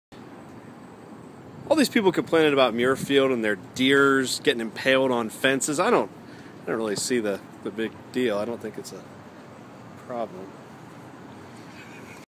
Deer caught on the fence?
Whats the big commtion about deer impaling themselves on the fences at Muirfield?